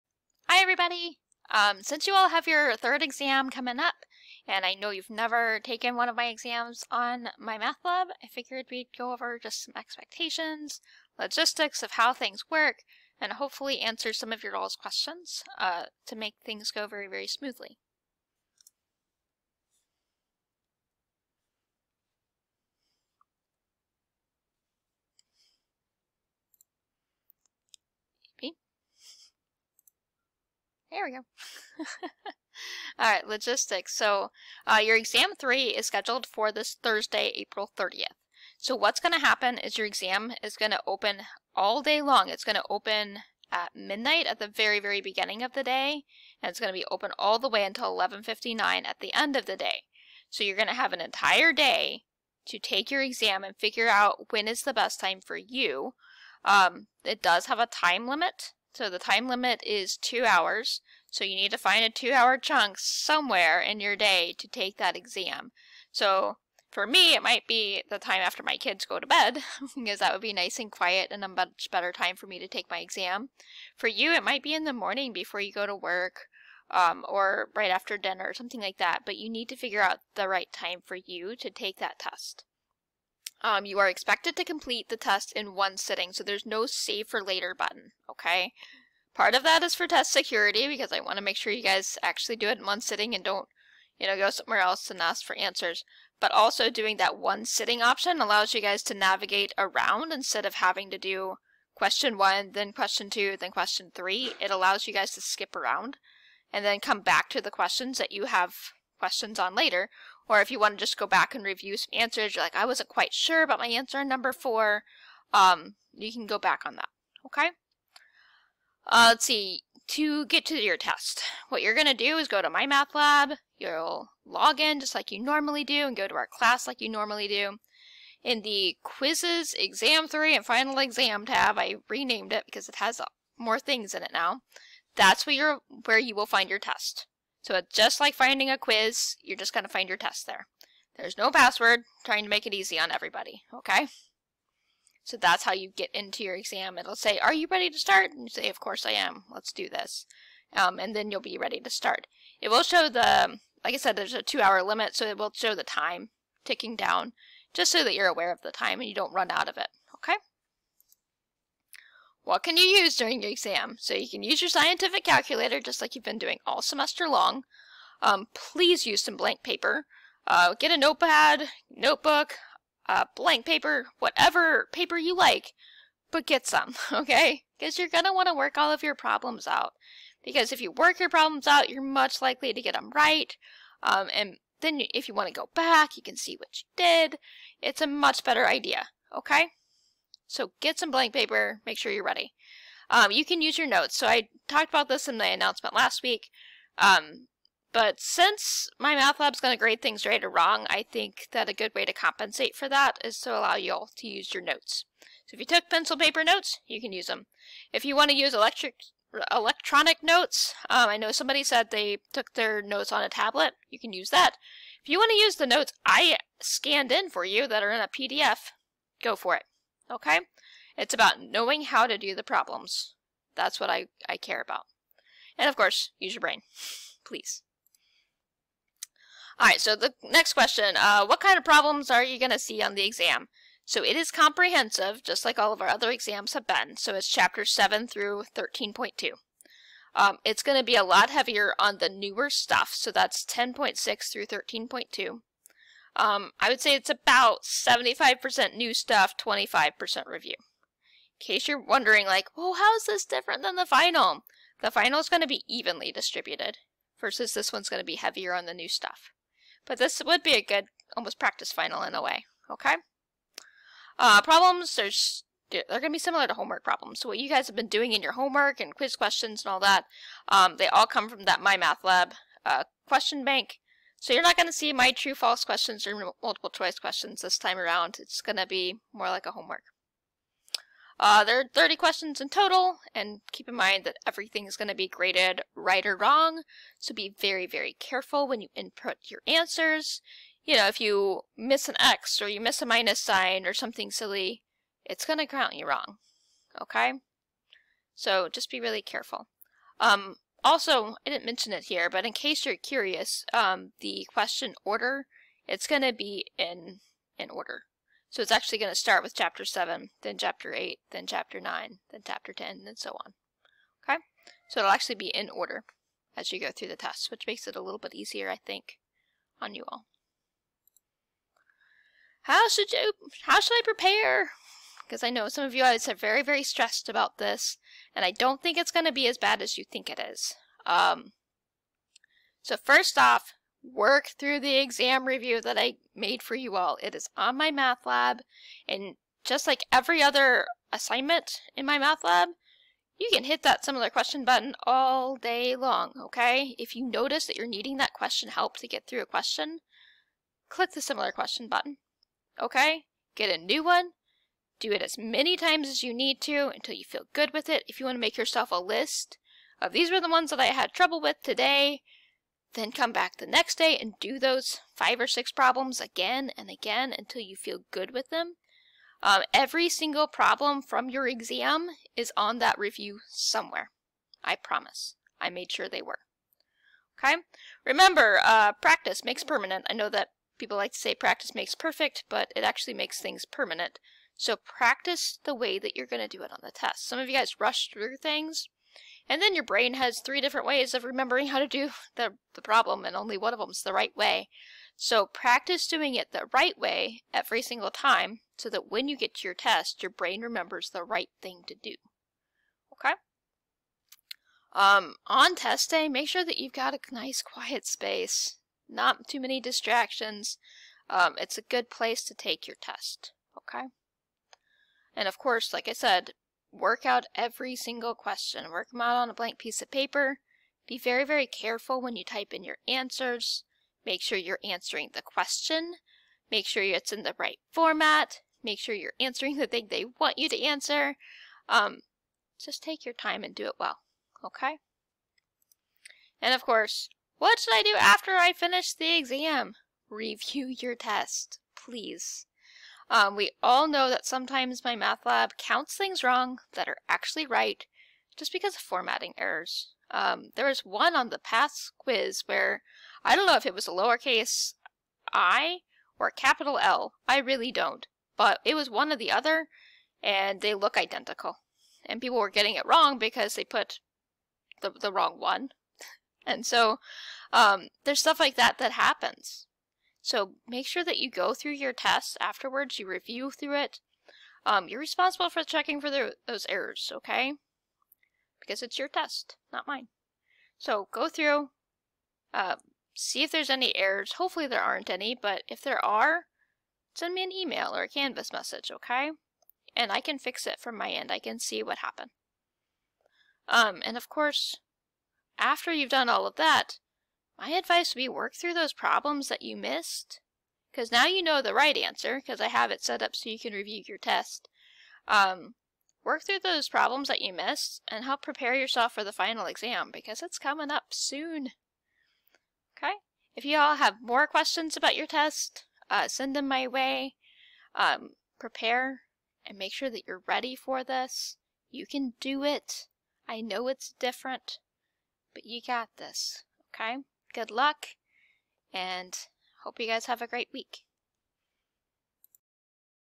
Exam #3 Expectations
Since going to Remote / Virtual Learning, our third Exam will be online. Here's a video about what to expect!